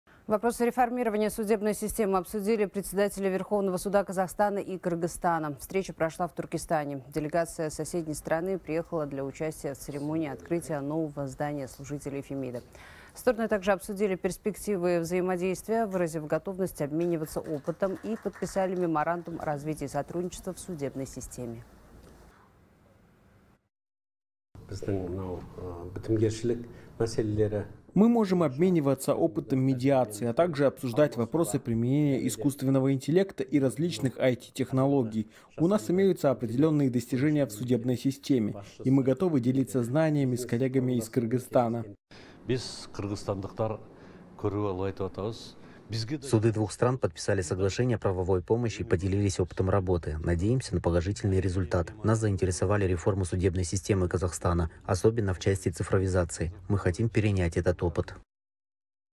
Меморандум о развитии сотрудничества в судебной системе подписали Казахстан и Кыргызстан
Вопросы реформирования судебной системы обсудили председатели Верховного Суда Казахстана и Кыргызстана. Встреча прошла в Туркестане, передаёт «24KZ».